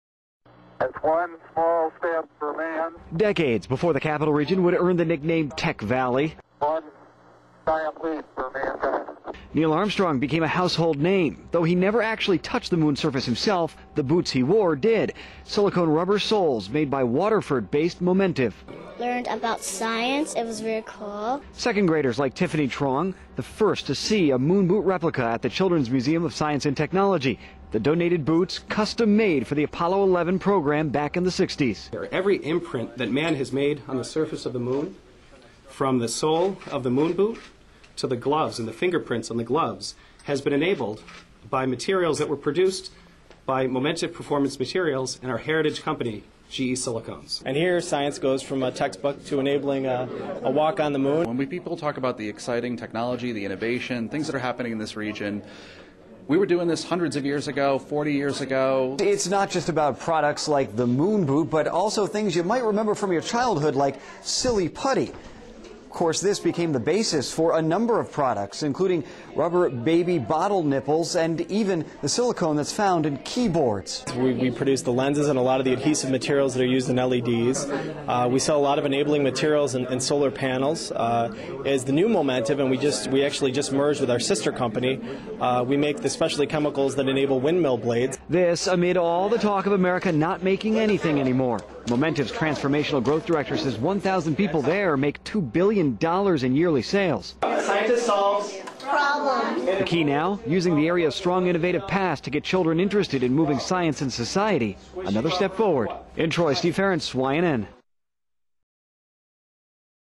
Moon-YNN-10-12-06.mp4
Coverage by local 24-hour news channel YNN (Your News Now) of the transfer of a moon boot prototype from Momentive Performance Materials to the Children's Museum of Science and Technology in Troy, NY